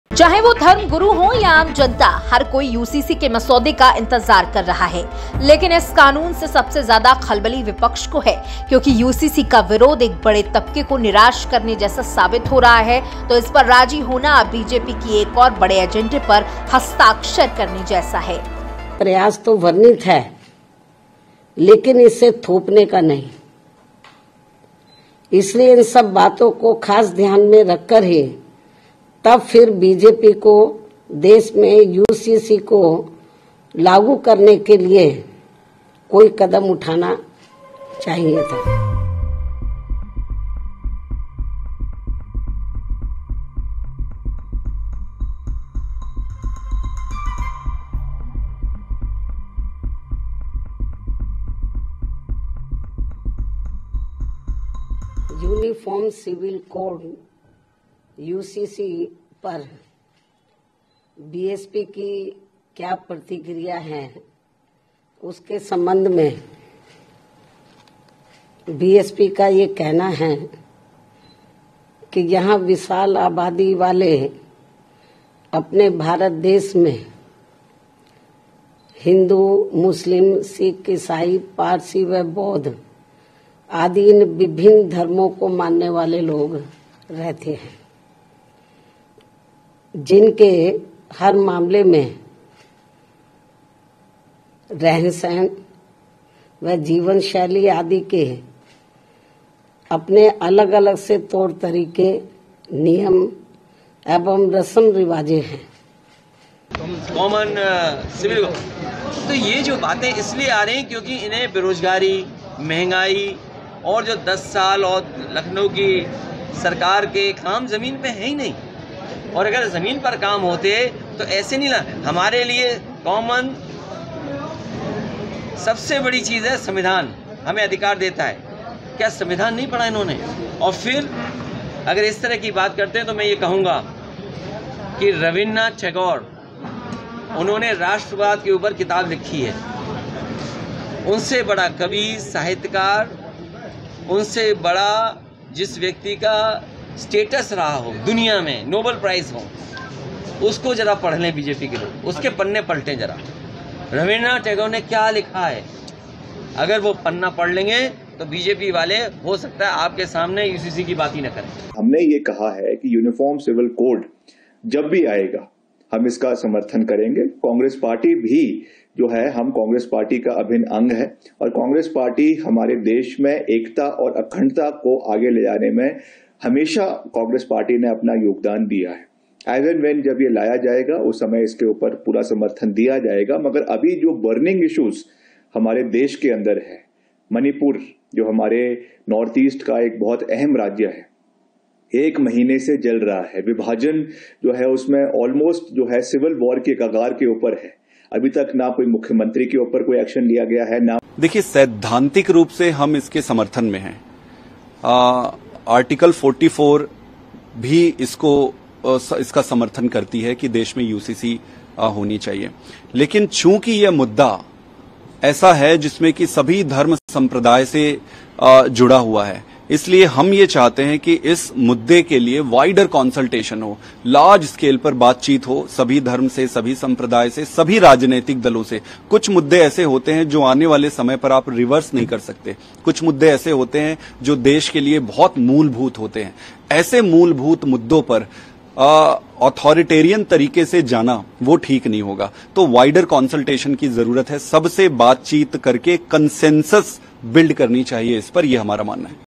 UCC के मुद्दे पर क्या कानून की आड़ में राजनीति चमका रहा विपक्ष?
चाहे वो धर्म गुरु हो या आम जनता हर कोई UCC के मसौदे का इंतजार कर रहा है..लेकिन इस कानून से सबसे ज्यादा खलबलि विपक्ष को है क्योकि UCC का विरोध एक बड़े तपके को निराश करने जैसा साबित हो रहा है तो इस पर राजी होना बीजेपी की एक और बड़े एजेंडे में हसताक्षर करने जैसा

Whether it is a religious leader or the general public, everyone is waiting for the draft of the UCC..but the opposition is the most disturbed by this law, because the opposition to the UCC is proving to be a disappointment to a big devotee, so agree on it Like signing another big agenda of BJP.

DEN- 157
SITI –85
JGD-99